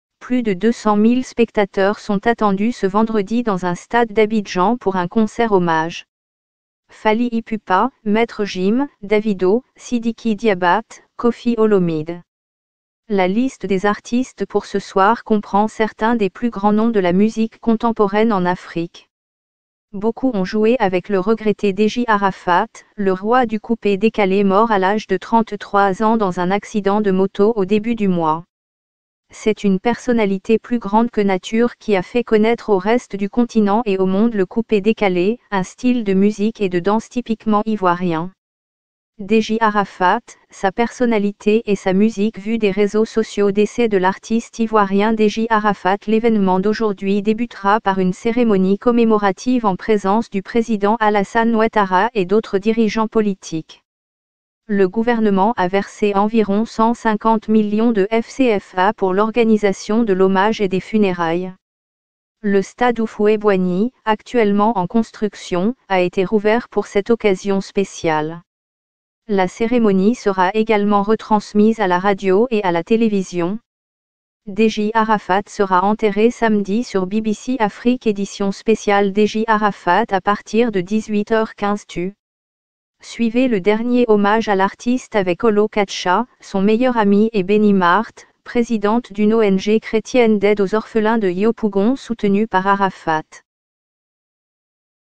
La Côte d'Ivoire s'apprête à faire ses adieux au célèbre chanteur DJ Arafat
Plus de 200 000 spectateurs sont attendus ce vendredi dans un stade d'Abidjan pour un concert hommage. Fally Ipupa, Maitre Gims, Davido, Sidiki Diabate, Koffi Olomide. la liste des artistes pour ce soir comprend certains des plus grands noms de la musique contemporaine en Afrique.